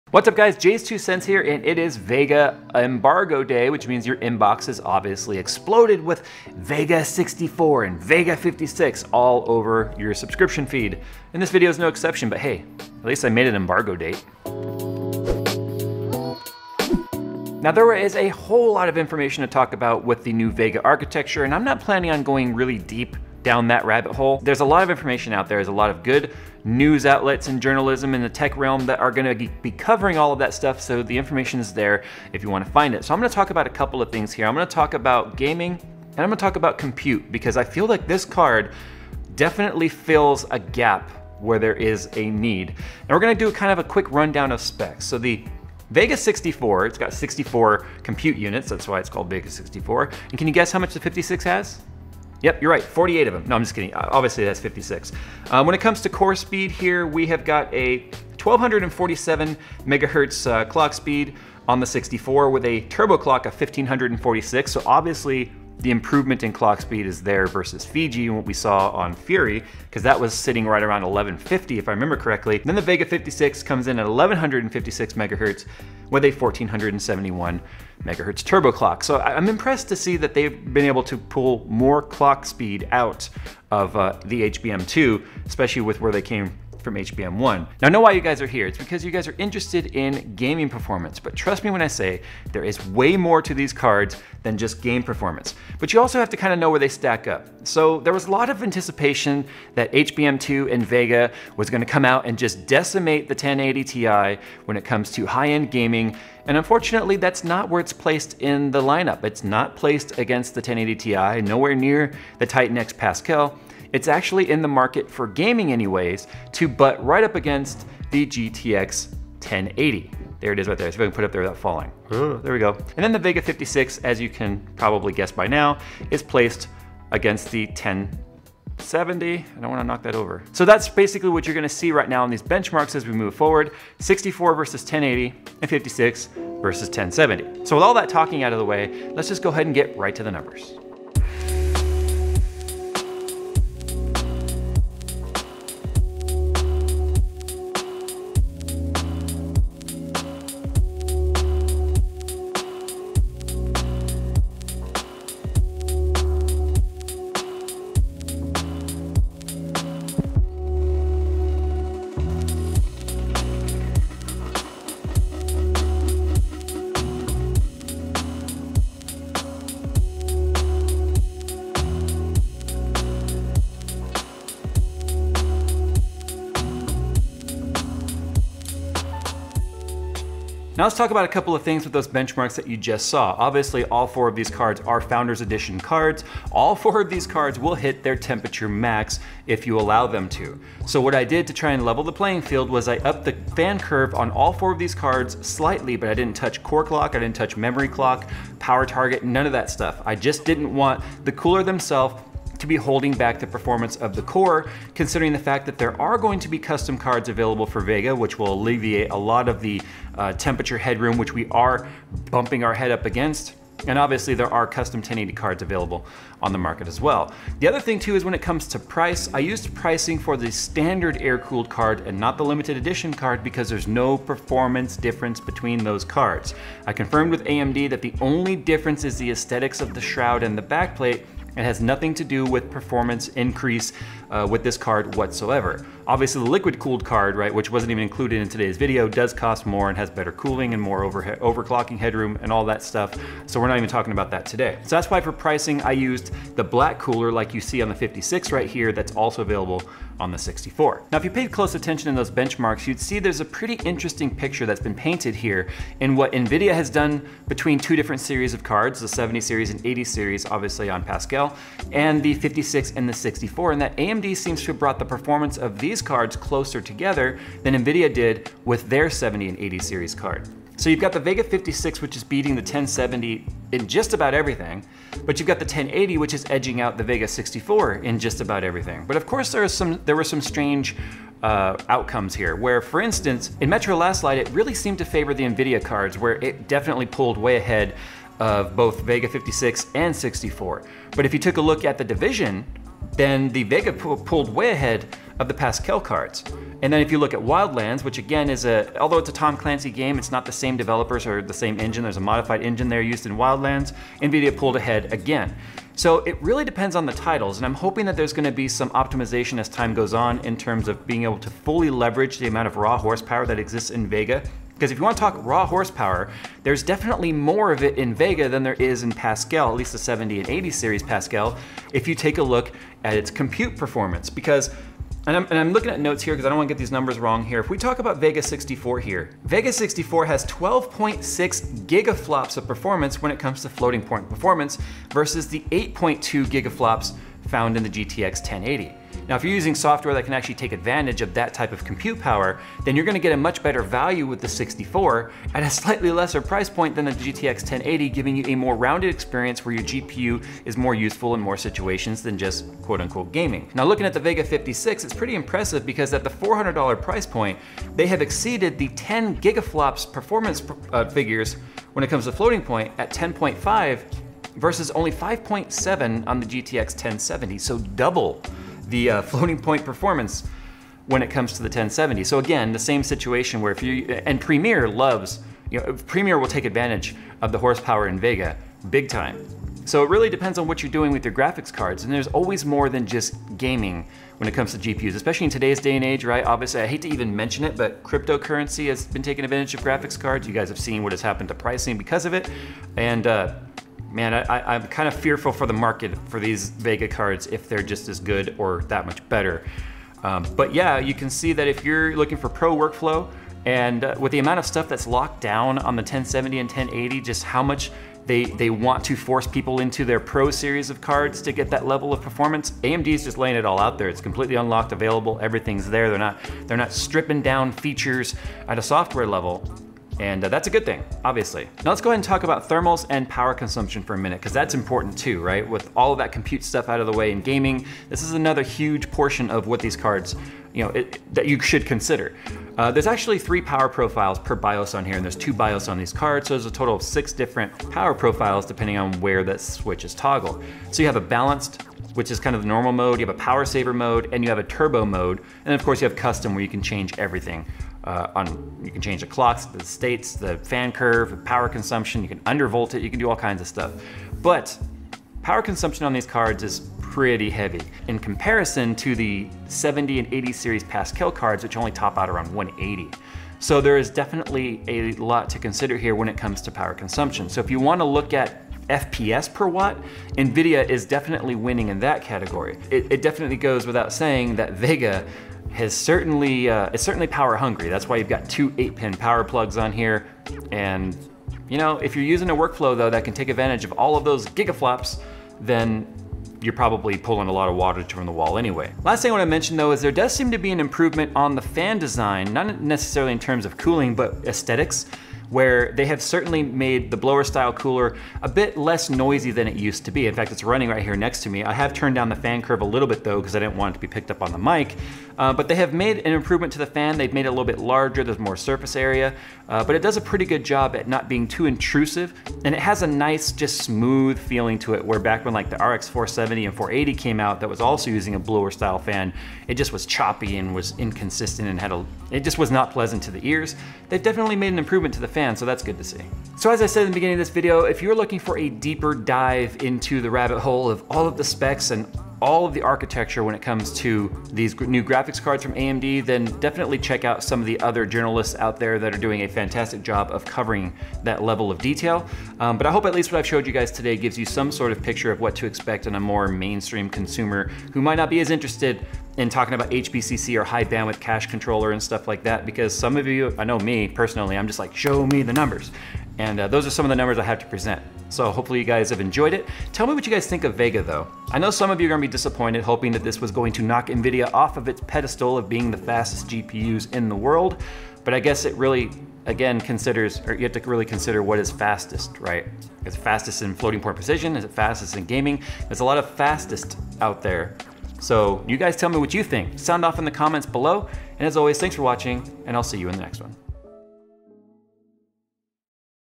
AMD RX Vega vs NVIDIA GTX for Gaming
AMD's highly anticipated Vega 56 and 64 GPUs are finally here. So how do they perform in games? Also note that I will be following up with 2 separate videos featuring Overclocking and Compute/Rendering performance soon!

○○○○○○ Items featured in this video available at Amazon ○○○○○○

► Amazon UK
► Amazon Canada

About JayzTwoCents